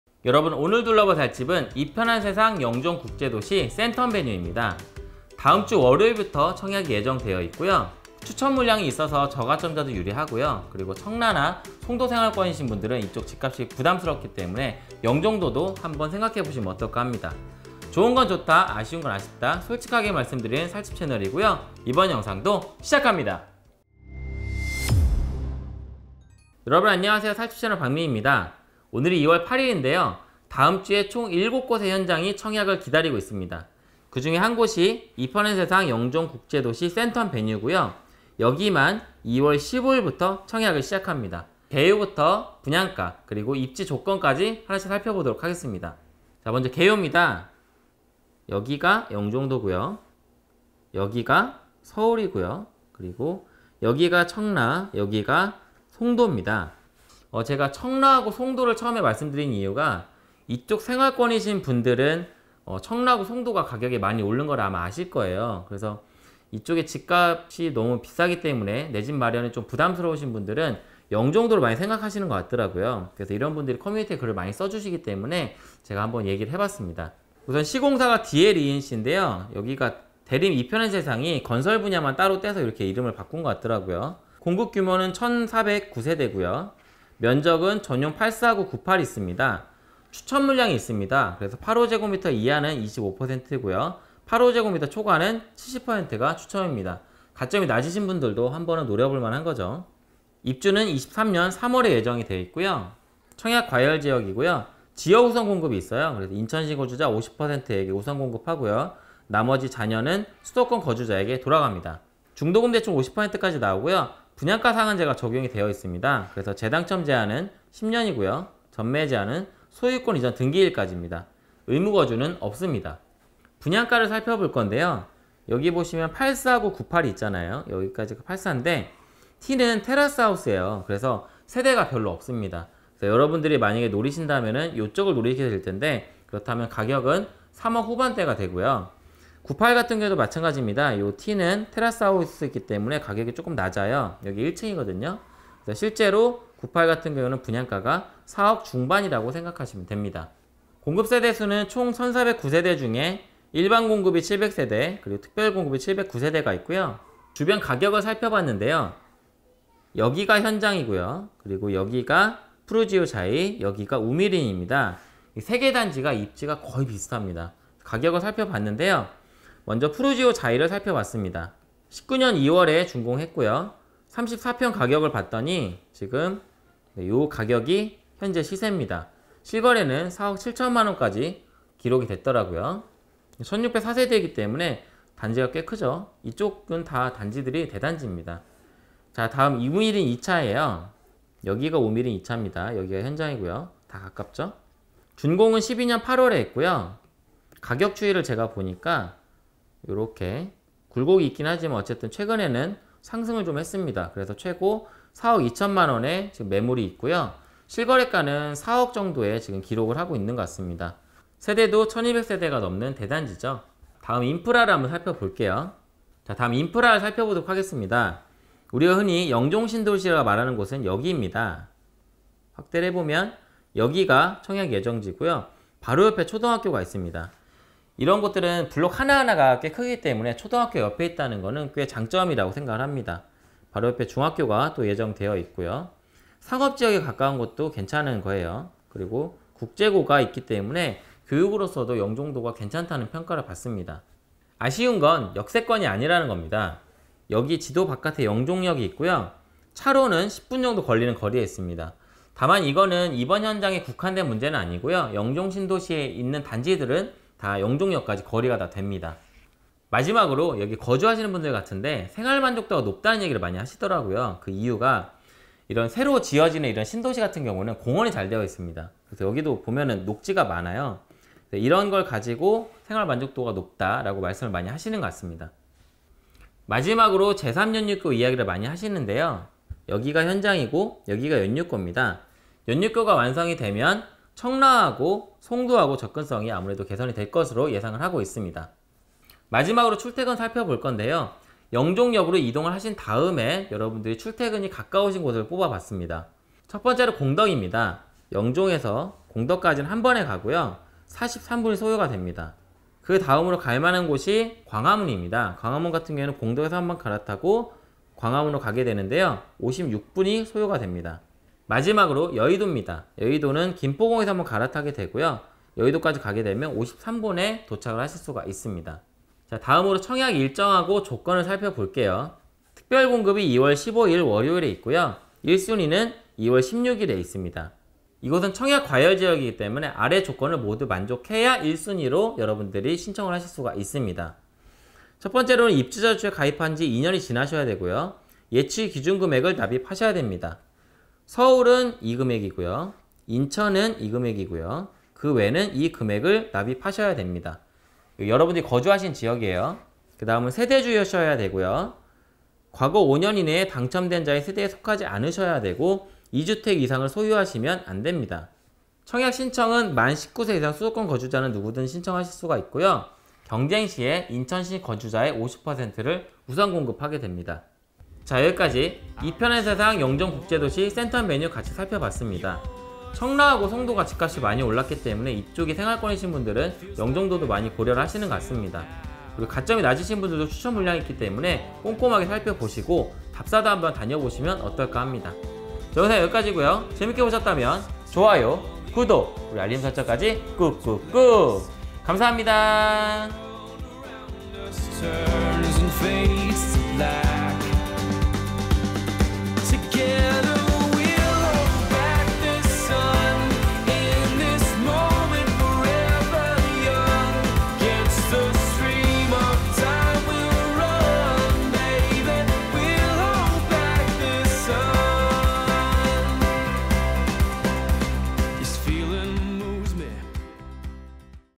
(SUB자막)송도 집값이 부담스럽다면? 3억대 분양가, e편한세상 영종국제도시 센텀베뉴 [살집채널 박미미]
살집채널은 객관적인 정보전달을 위해 노력합니다